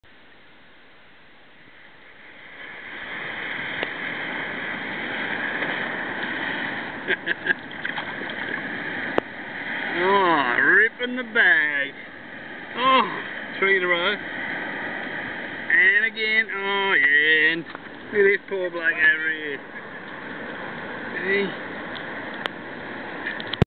Indo Surfing Drop In
Blatant Drop in at a remote Sumatran surf spot - well done Sam!